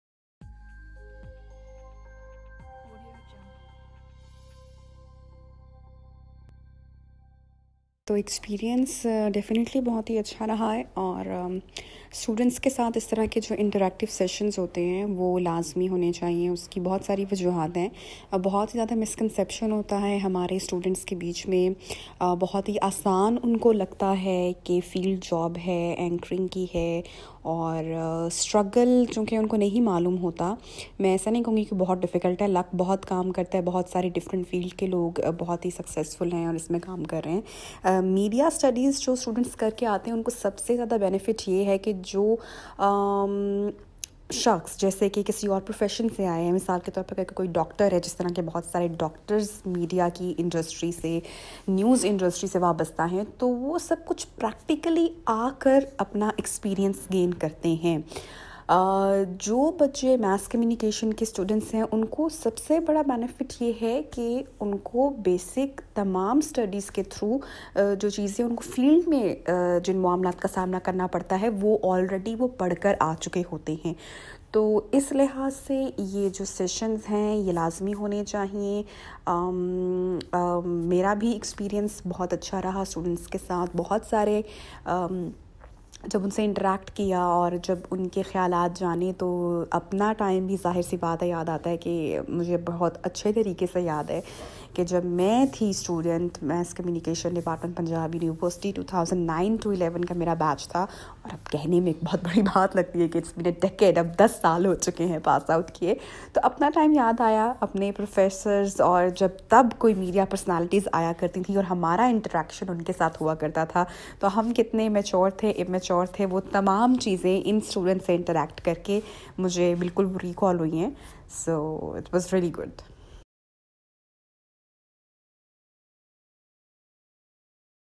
Nabeeha Ejaz talks about avenues for media students in the industry
Media is not all about anchoring or hosting , it Is much more than that: Nabeeha Ejaz talks about her workshop with media students at FC College, Lahore